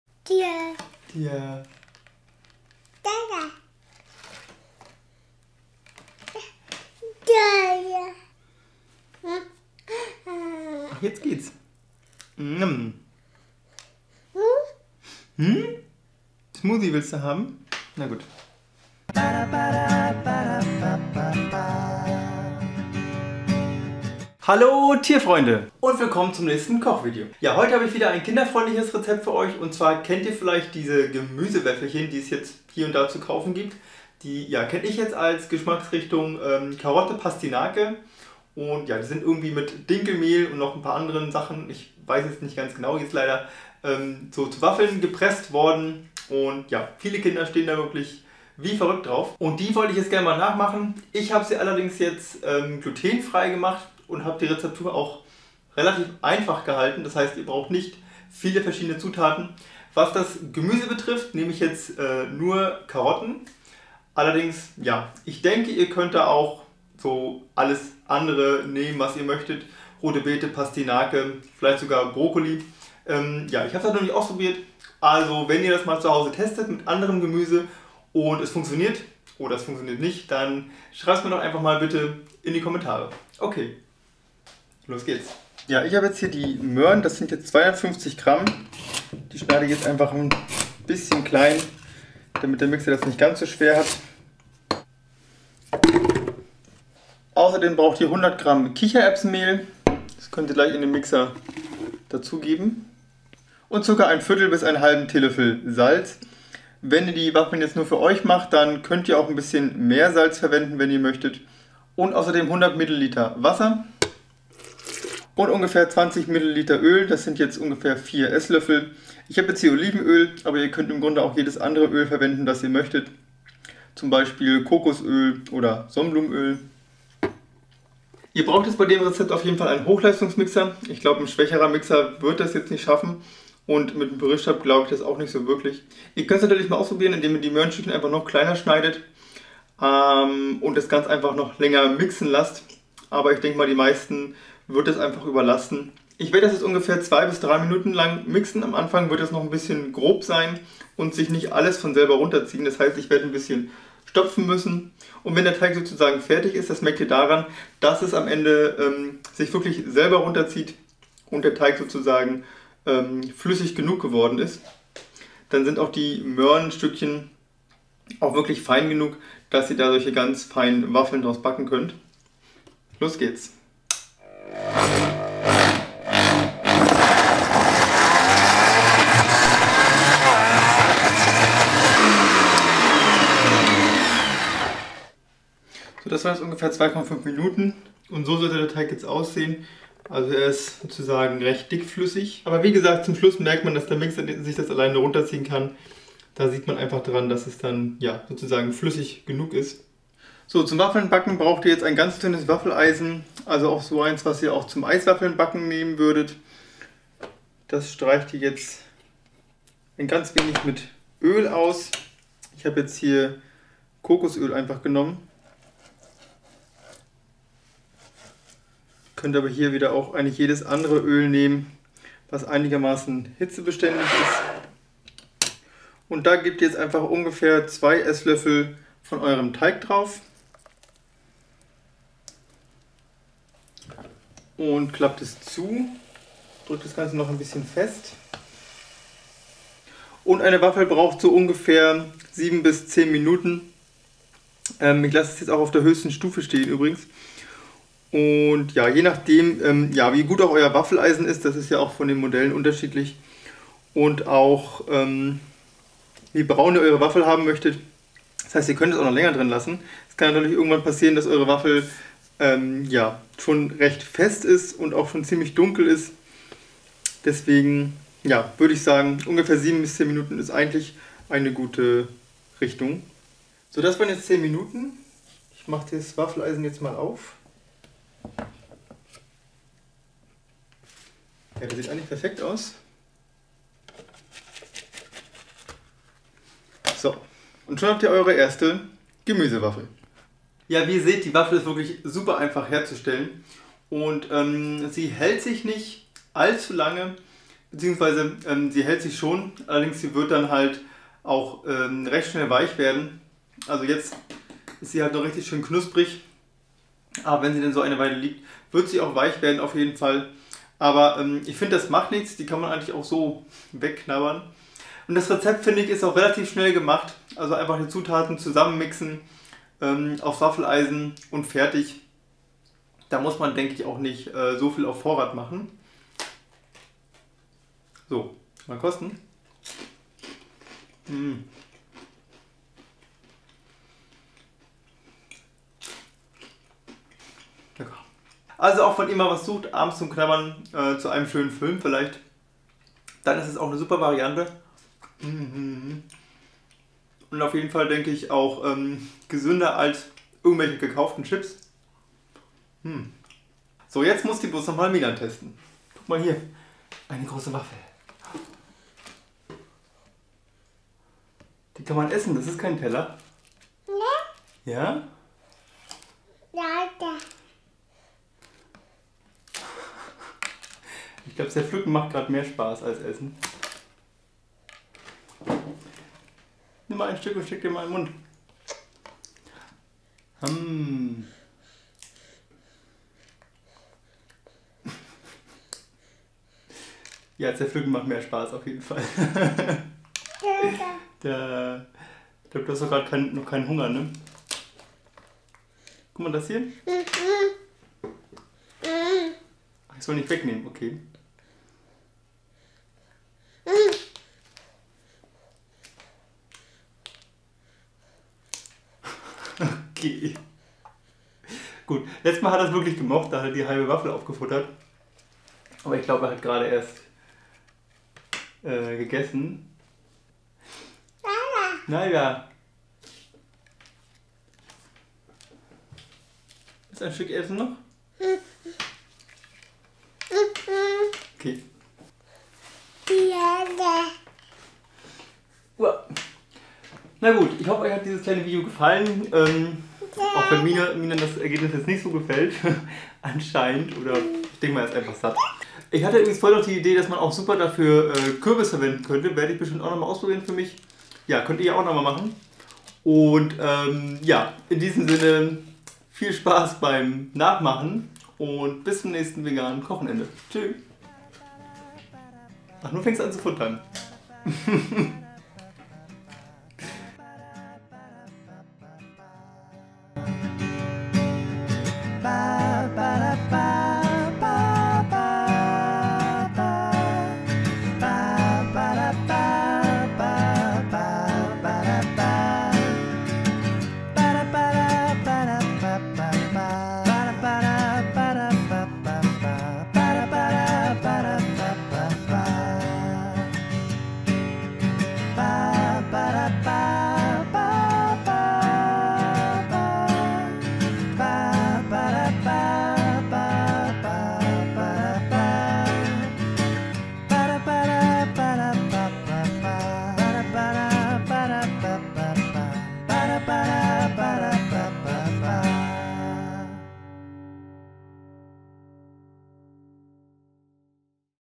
Gemüsewaffeln - Vegane Kinderrezepte für Kindergeburtstag - Kinder Waffeln Rezept - Koch's vegan
Dieses mal gibt's ein leckeres Gemüsewaffelrezept. Geliebt von allen Kindern (naja fast allen ;D ). Wenn eure Kleinen, oder auch ihr, mal wieder Lust auf einen kleinen Snack oder etwas zum Knabbern habt und nicht zur Chipstüte greifen wollt, dann probiert doch mal diese selbstgebackenen Waffeln. Super einfach hergestellt aus einer Zutatenliste, die sich an einer Hand abzählen lässt^^. Variationen in alle Gemüserichtungen möglich ;) Wenn ihr noch gute Ideen für selbstgebackene Gemüsewaffeln habt, dann schreibts doch mal in die Kommentare. Ich freue mich auf Anregungen und alle anderen bestimmt auch. Bis dann und frohes Knabbern. :)

Zutaten:
250 g Möhren,
100 g Kichererbsenmehl
1/4 - 1/2 TL Salz
100 ml Wasser
20 ml Öl

etwas Kokosöl zum Einfetten